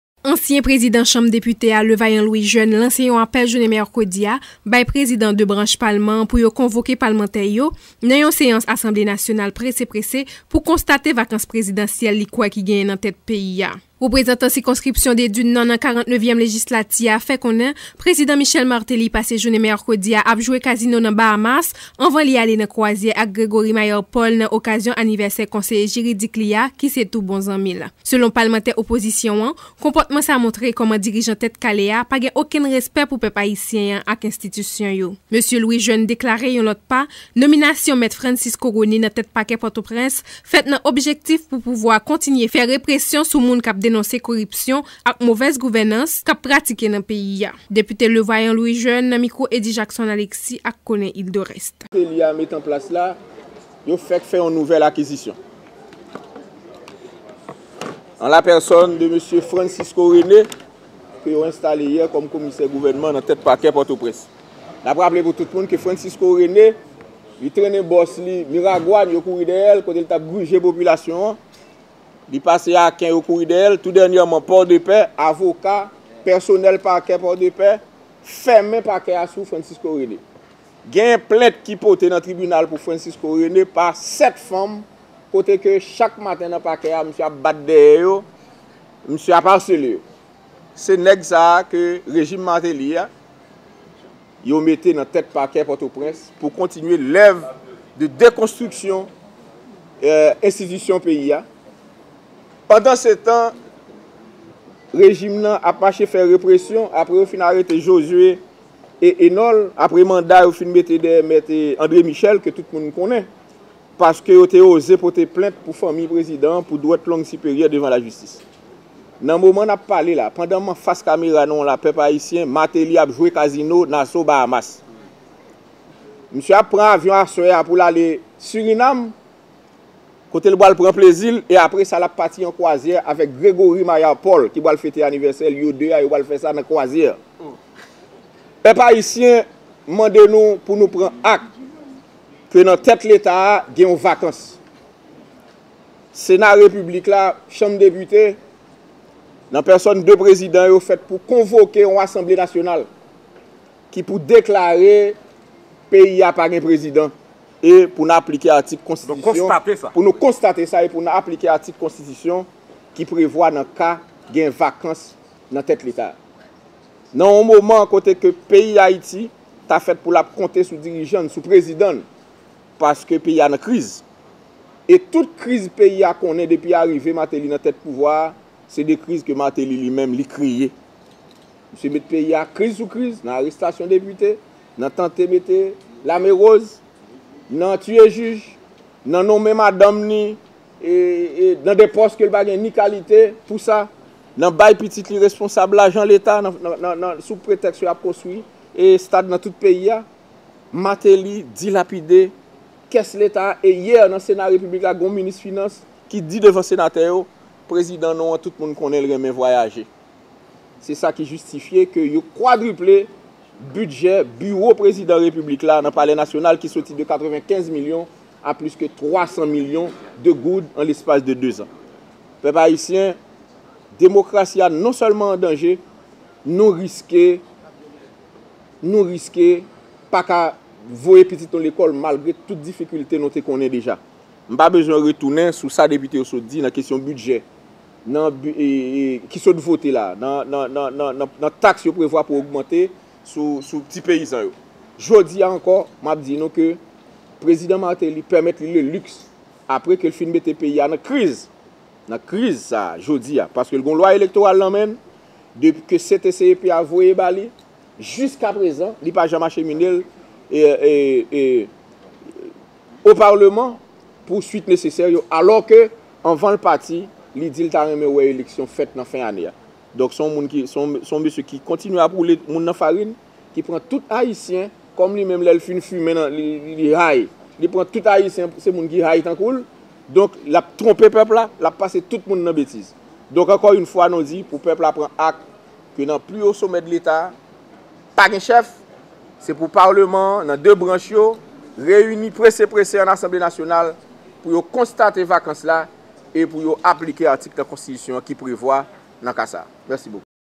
Le député Levaillant Louis-Jeune réclame la convocation d'une Assemblée Nationale
Michel Martelly jouait mercredi dans un casino, aux Bahamas, selon Levaillant Louis-Jeune qui invite ses pairs à constater la « vacance présidentielle ».

Ulcéré, le Député de Desdunes exhorte les présidents des deux Chambres à convoquer en urgence une séance en assemblée nationale afin d'enclencher la procédure de destitution du Président-chanteur qui aurait abandonné son pays au milieu du gué pour se la couler douce à l'étranger, en prenant notamment part à une croisière pour l'anniversaire de son conseiller et ami, Grégory Mayard Paul

L'ancien président de la Chambre basse, Levaillant Louis-Jeune, a appelé mercredi les deux Chambres du Parlement à se réunir d'urgence afin de statuer en assemblée nationale sur la « vacance présidentielle » provoquée selon lui par Michel Martelly alors que le chef de l'Etat a, ces derniers jours, laissé un pays au bord de la crise pour un séjour de deux semaines à l'étranger.

Considéré depuis quelque temps comme l'un des tenants de l'opposition radicale au régime Tèt Kale (crâne rasé), le représentant de la circonscription de Desdunes (Artibonite, nord) s'est adressé aux présidents du Sénat, Simon Dieuseul Desras, et de la Chambre des Députés, Jean Tholbert Alexis, pour dénoncer l'irresponsabilité du Président haïtien qui serait en train de se payer du bon temps dans les Caraïbes.

L'ancien chanteur de Compas surnommé « Sweet Micky » aurait ainsi passé sa journée de mercredi à jouer au casino dans l'archipel des Bahamas, a révélé le Député Louis-Jeune qui dit tenir cette information de sources crédibles. Le voyage de plaisance du chef de l'Etat devrait se poursuivre par une croisière en compagnie de Me Grégory Mayard Paul dans le but de célébrer, comme il se doit, l'anniversaire de naisance de son conseiller juridique et bras droit.

Un comportement aussi indigne montre à quel point M. Martelly n'a « aucun respect pour le peuple haïtien et les institutions du pays », s'est indigné le parlementaire qui, à maintes reprises, a souhaité que la procédure de destitution du Président soit lancée.

Par ailleurs, Levaillant Louis-Jeune inscrit la fraîche nomination à la tête du parquet de la capitale de Francisco René dans le cadre de la campagne de répression orchestrée, dit-il, contre ceux qui dénoncent la corruption et la gabegie administrative prévalant au sein de l'équipe Martelly/Lamothe.

Parti incognito de Port-au-Prince -apparemment pour de belles vacances- le dirigeant haïtien est attendu ce week-end au Suriname où il doit asssiter à la clôture de la 11e édition du festival culturel des Caraïbes CARIFESTA.